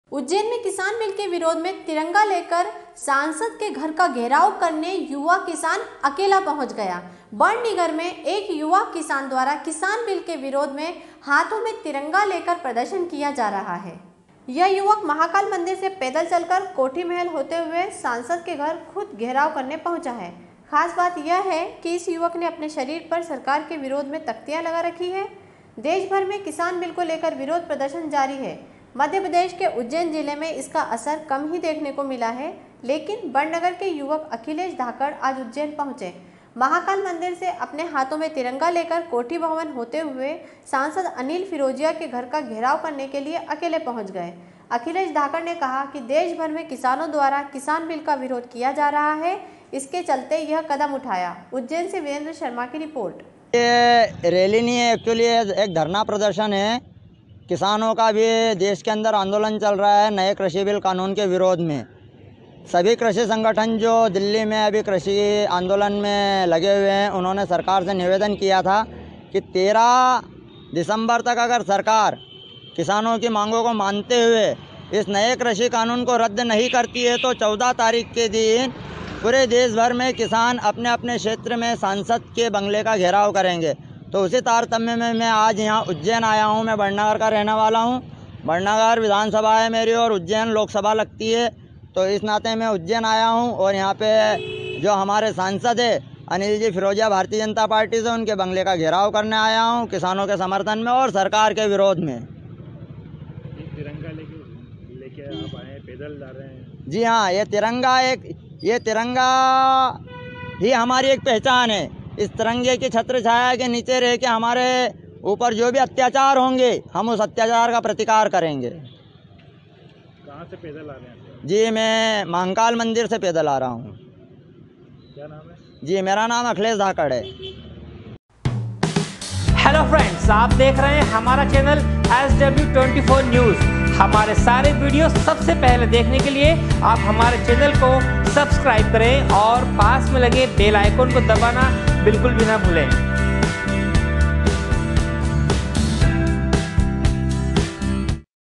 Ujjiain- बड़नगर के एक युवा किसान बिल के विरोध में हाथों में तिरंगा लेकर प्रदर्शन किया जा रहा है।
Ujjiain- SW 24 News Madhyapradesh ujjain SW24news #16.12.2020
उज्जैन में किसान बिल के विरोध में तिरंगा लेकर सांसद के घर का घेराव करने युवा किसान अकेला पहुंच गया। बड़नगर के एक युवा किसान द्वारा किसान बिल के विरोध में हाथों में तिरंगा लेकर प्रदर्शन किया जा रहा है। उज्जैन से वीरेंद्र शर्मा की रिपोर्ट